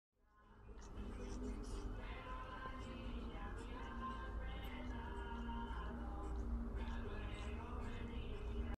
Gata cuida perro
Siempre se ha dicho q los gatos y los perros no se llevan bien o que los perros son más cariñosos que los gatos, para los q aún lo crean estos son Phoebe y Fito. Parte 1